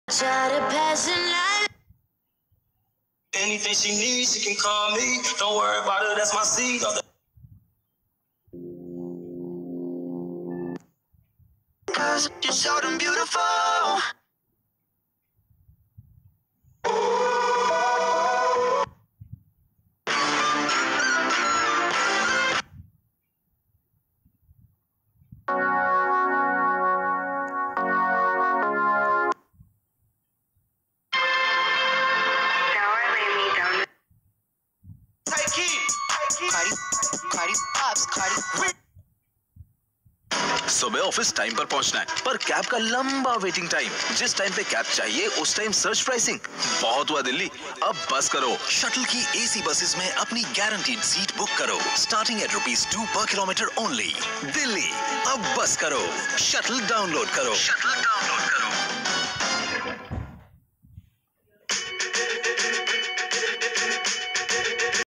Shuttl Jun19 Audio Ad Campaign | Paytunes Audio Advertising - Spotify, Gaana, Jio Saavan, & more
Along with spending its media budget on the typical advertising options like TV and Radio, Shuttl —along with PayTunes—has taken the Digital audio advertising route for this campaign.

The Digital audio campaign is titled Shuttl

With the impressive growth of the Digital audio advertising industry, it has become imperative that you target the right audience, right moment to catch the consumer, as done in this campaign.

CREDITS
Client: Shuttl
DSP: PayTunes

Audio Ads
Audio Advertisers
Audio Ads in India
Audio Ad campaigns in India.